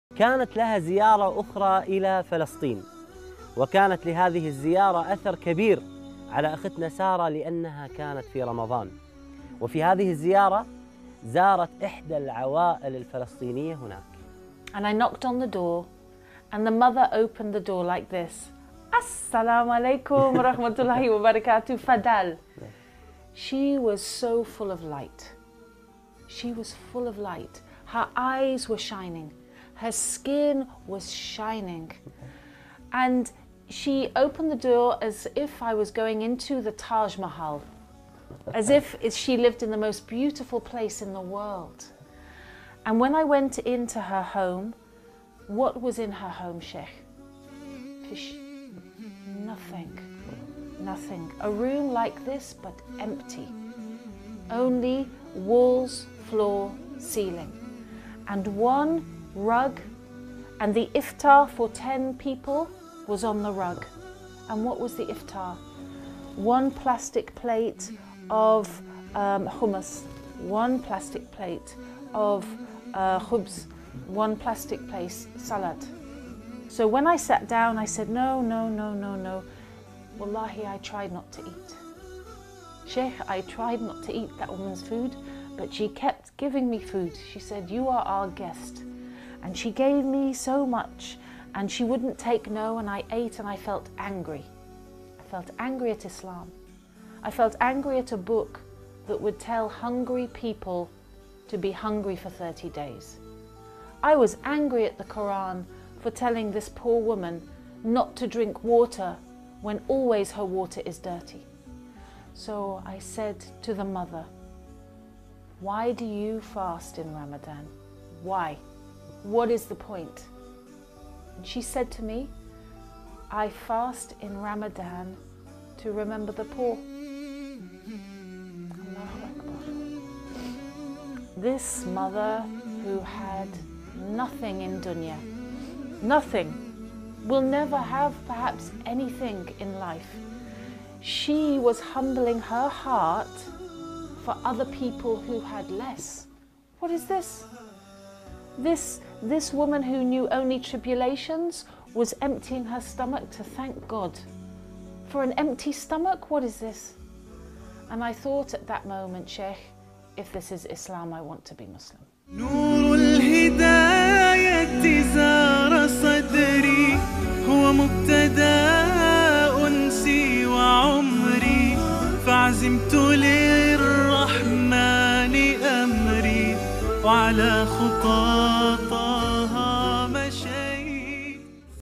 Lauren Booth - Tony Blair's sister converted to Islam because of a poor Muslim woman in Ramadan
Lauren Booth - Tony Blair's sister converted to Islam because of a poor Muslim woman in Ramadan  in Palestine
 أسلمت بسبب امرأة مسلمة فقيرة في رمضان